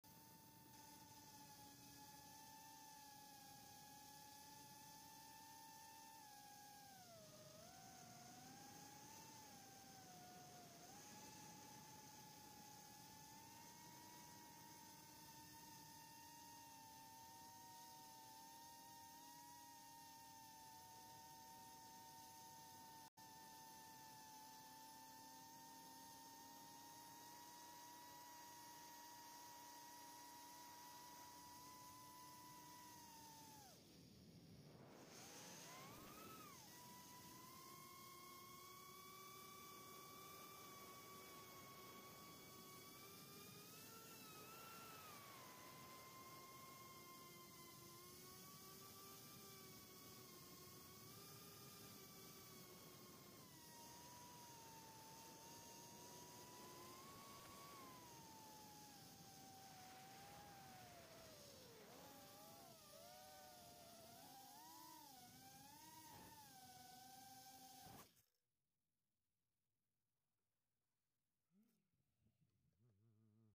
Through the trees and above
A couple of moments flying during my trip to NH this summer.